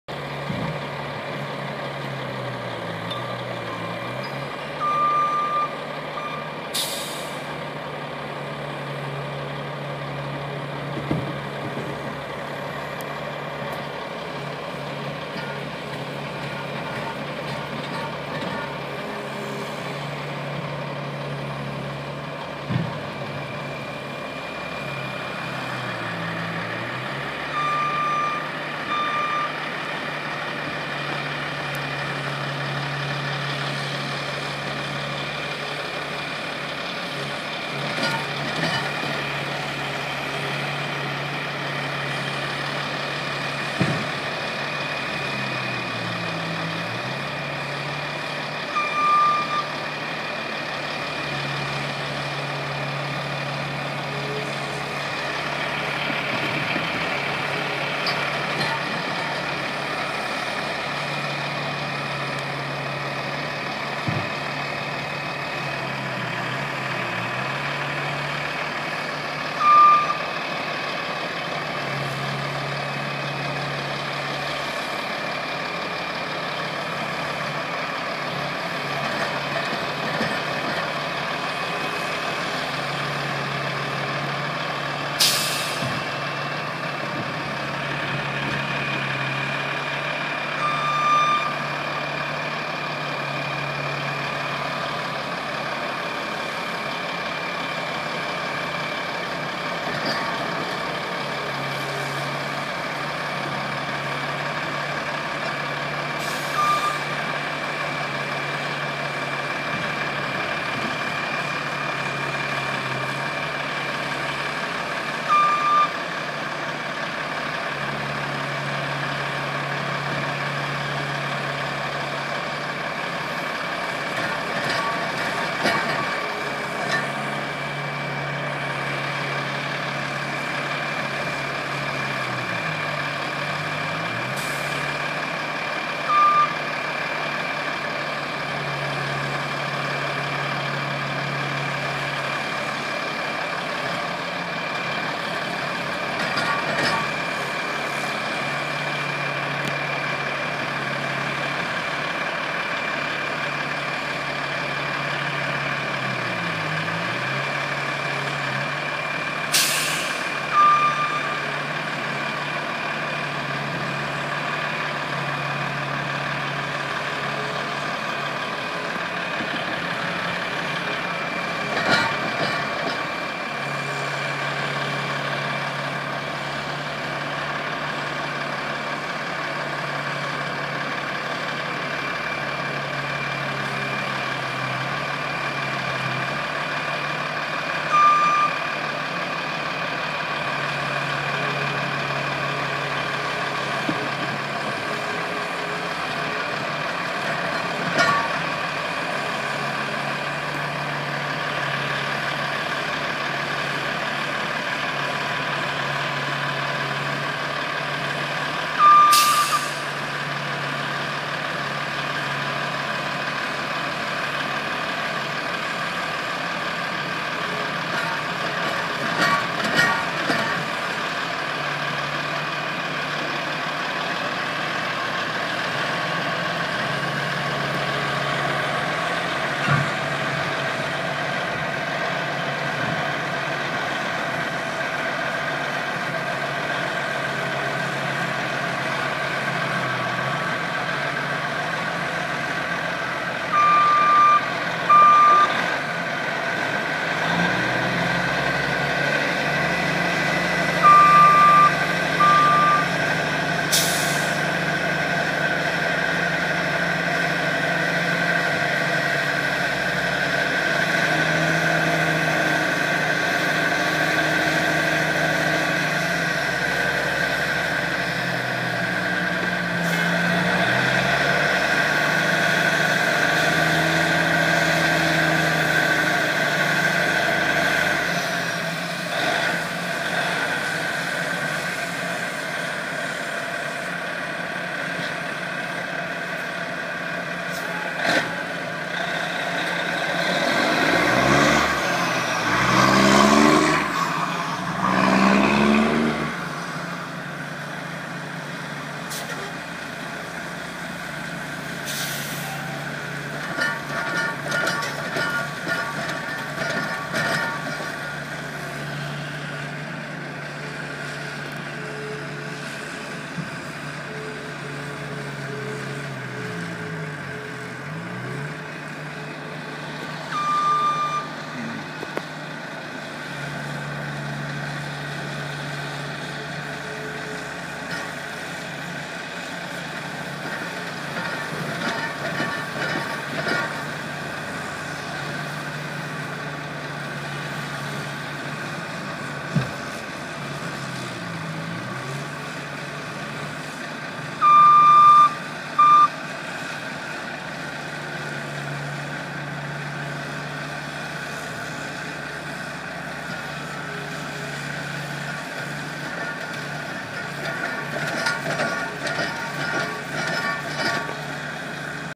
Snow Hauling by Josh Rutt
Josh Rutt Snow Removal Services hauling snow from a parking ramp.